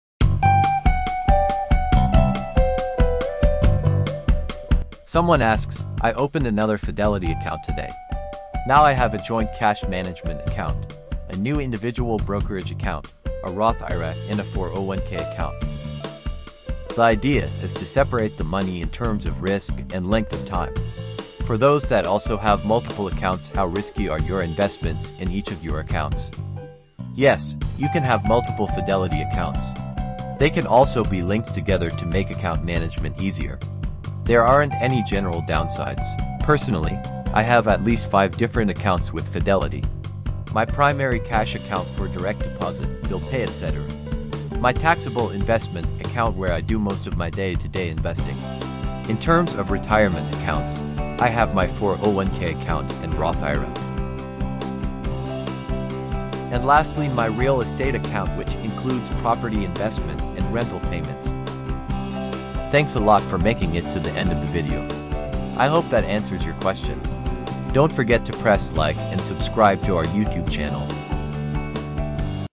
How Many Fidelity Accounts Can I Have? Here's the Truth!
You can have more than one account, and you can open multiple Fidelity accounts at any time.